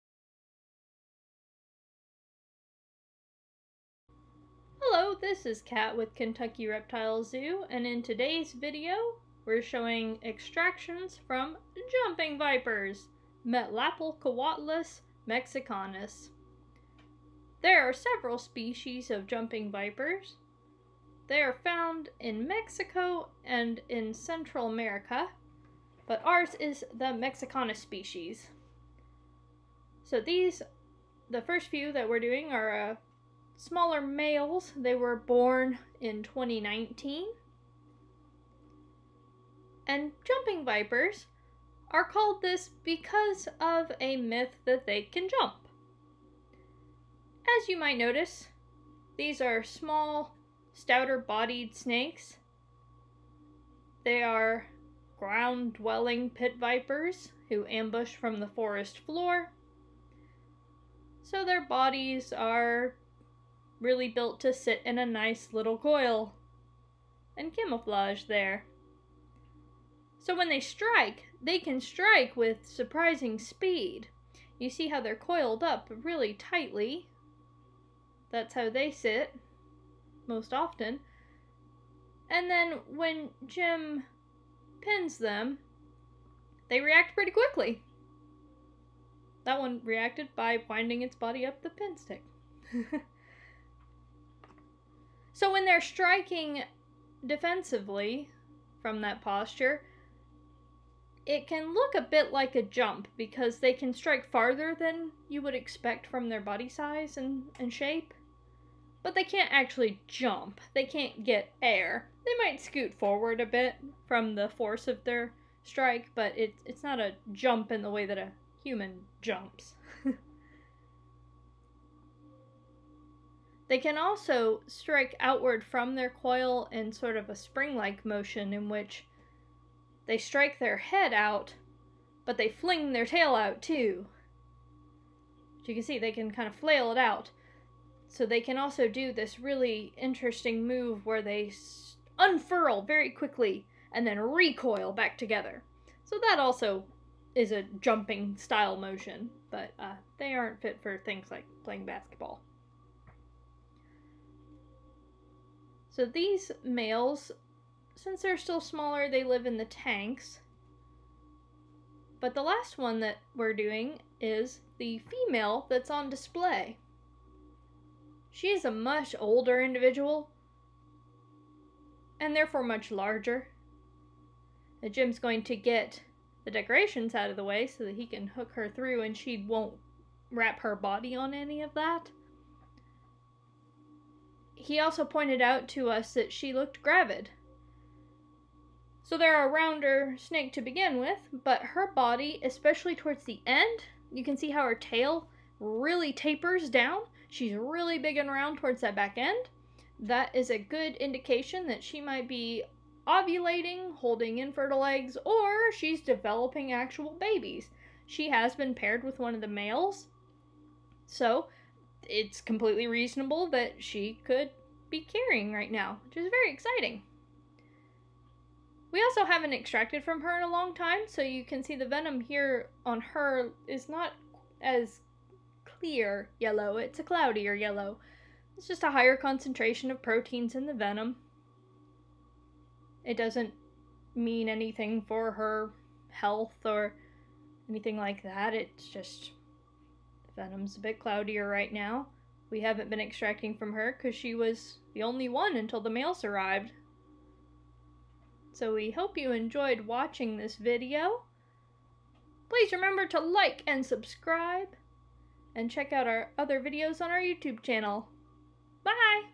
JUMPING vipers?! Yep, venom extraction from Jumping Vipers
They don't really jump, but it can look like they do! An explanation of their interesting behaviors, and venom extraction from these vipers. Metlapilcoatlus mexicanus.